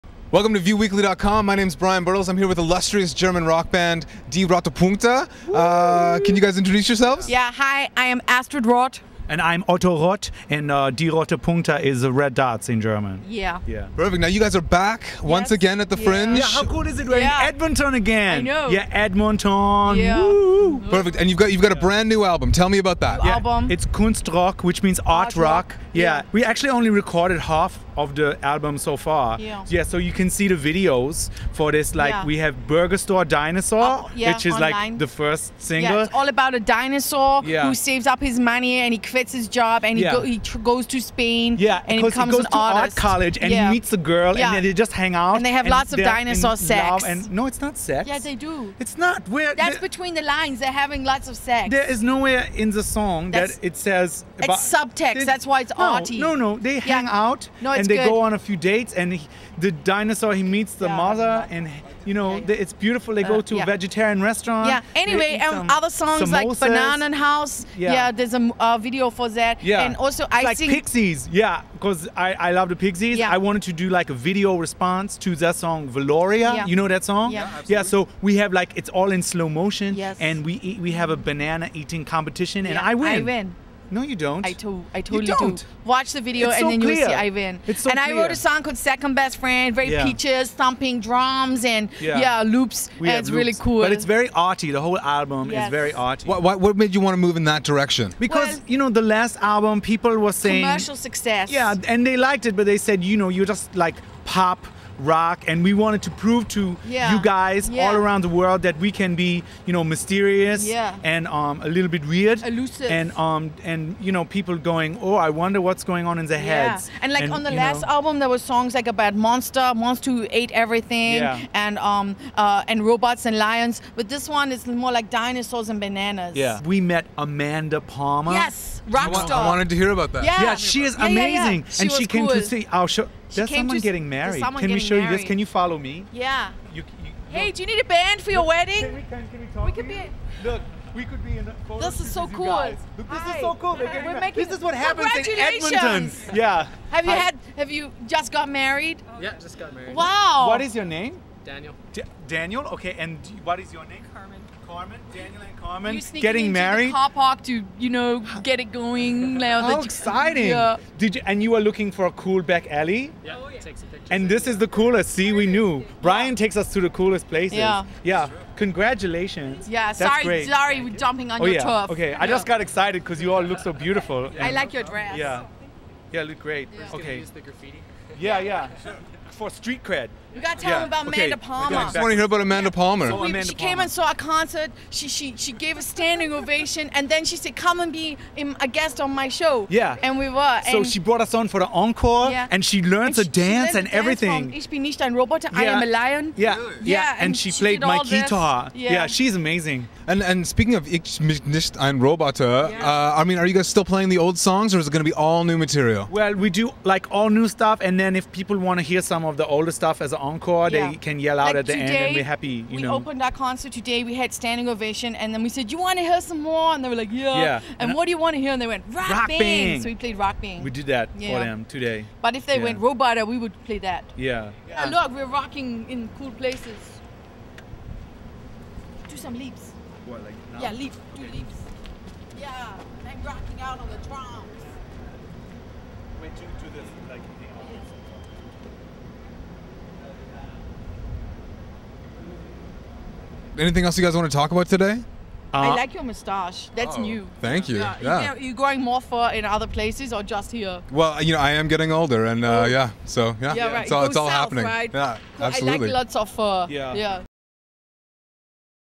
Vue Weekly: Edmonton Fringe Festival 2010 - "Die Roten Punkte"
Die Roten Punkte talk to Bryan Birtles about their new direction and returning to the Edmonton Fringe Festival.